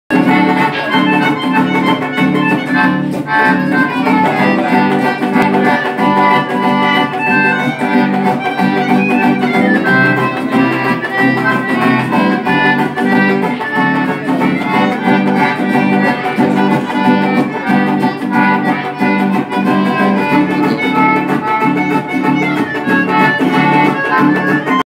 Boere musiek Four Seasons Stilbaai
17 februari 2018. Die manne speel daarem lekker
Met Reinier Oosthuizen op s'n Concertina